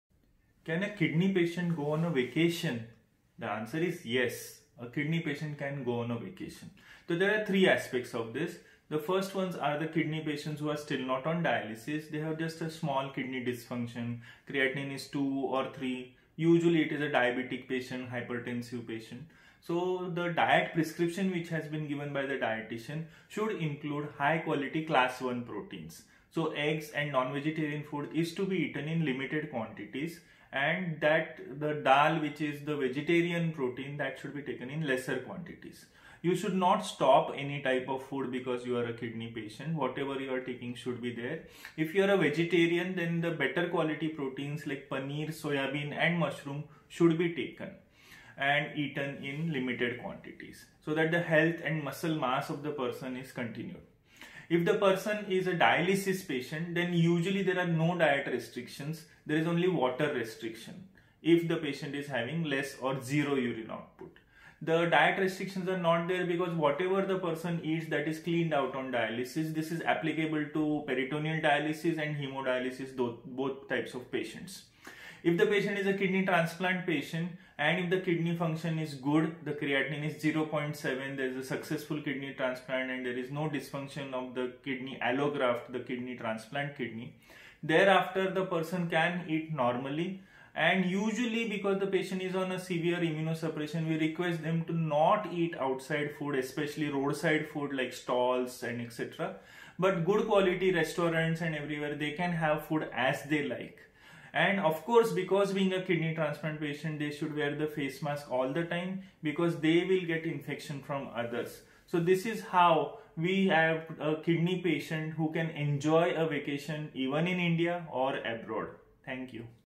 Kidney Patient can go on a Vacation Not on Dialysis or Even on Dialysis
Can a Kidney Patient go on a Vacation? The answer is, Yes. A Kidney Patient can go on a Vacation. So, there are 3 aspects of this.
The First ones, are the Kidney Patients, who are still not on dialysis. They have just a Small Kidney Dysfunction. Creatinine is 2 or 3. Usually, it is a  Diabetic Patient, Hypertensive Patient. So, the Diet Prescriprion, which has been given by the Dietician, should include, High Quality Class 1 Proteins. So, Eggs, and, Non-vegeterian Food is To Be Eaten, in Limited Quantities. And, that the, Dal, which is, the Vegeterian Protein, that, should be taken in lesser Quantities. You should Not Stop any type of Food, because you are a Kidney Patient. Whatever you are taking,  should be there. If, you are a  Vegetarian, then, the Better Quality proteins, like Paneer, Soyabean, and Mushroom, should be taken, and, Eaten in Limited Quantities. So, that, the Health and Muscle Mass, of the person, is continued.
If the Person is a Dialysis Patient, then usually, there are No Diet Restrictions. There is only Water Restriction, if the Patient is having Less, or, Zero Urine Output. The Diet Restrictions are not there, because, whatever the person eats, that is, Cleaned Out on Dialysis. This is applicable to Peritoneal Dialysis, and Hemodialysis, Both Types of Patients.
If the Patient is a Kidney Transplant Patient, and, if the Kidney Function is Good, the Creatinine is 0.7, there is a  Successful Kidney Transplant, and, there is No Dysfunction of the Kidney Allograft, the kidney, Transplant Kidney, thereafter, the person can eat Normally. And, usually, because, the Patient is on a Severe Immunosuppression, we request them, to Not eat Outside Food, especially, Roadside Food, like Stalls, and, etc.. But, Good Quality Restaurants, and, everywhere, they can have Food, as they Like. And, Of Course, because, being a  Kidney Transplant Patient, they should Wear a Face Mask all the time, because, they will get Infections from Others. So, this is how, we have a Kidney patient, who can enjoy a Vacation, even in  India, or Abroad. Thank You.